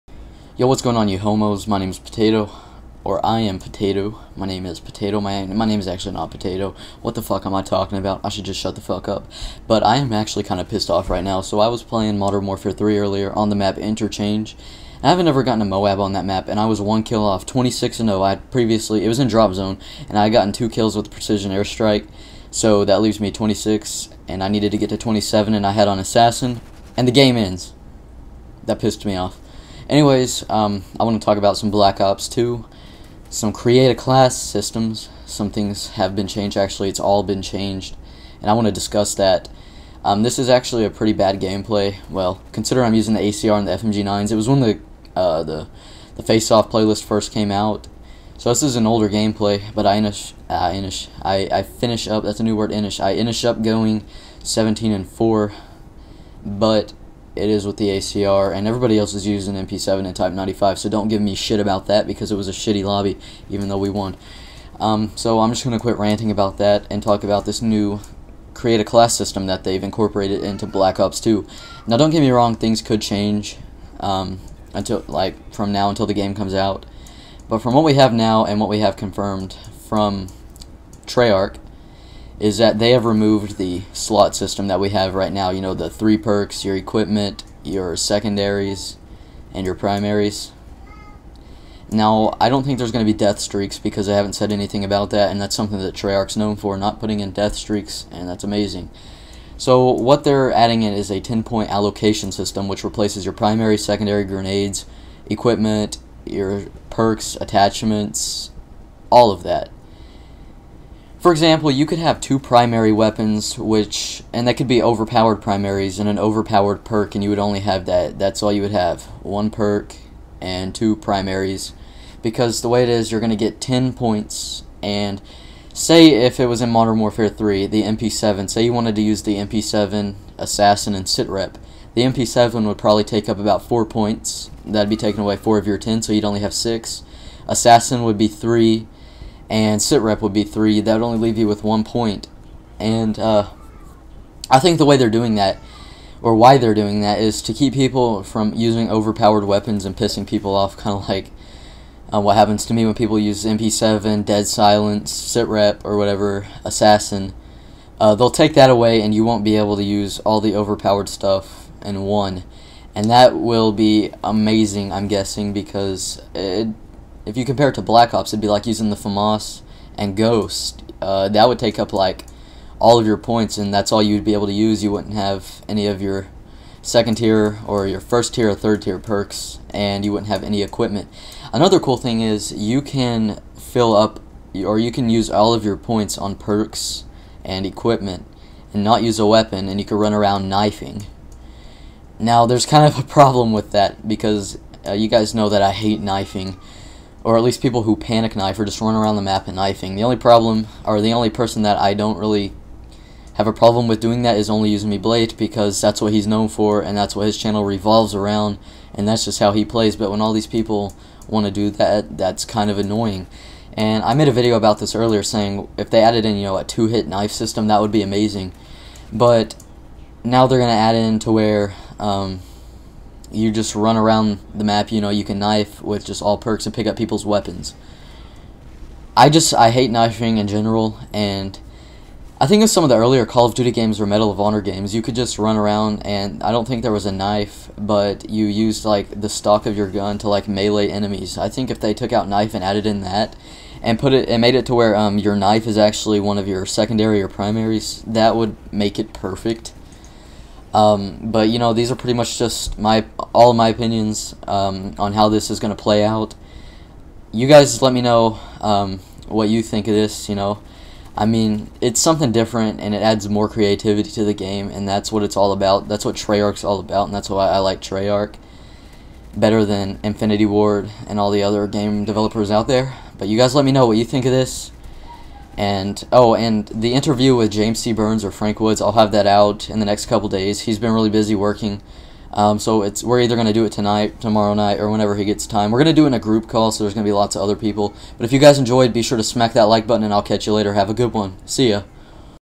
"Black Ops 2" New Create A Class System + Combat Training Confirmed!
Can we push for 15 likes again??

The create a class system for Black Ops 2 will be like anything we have seen before! Allowing
me creativity to the game! Also conbat training was confirmed by treyarch.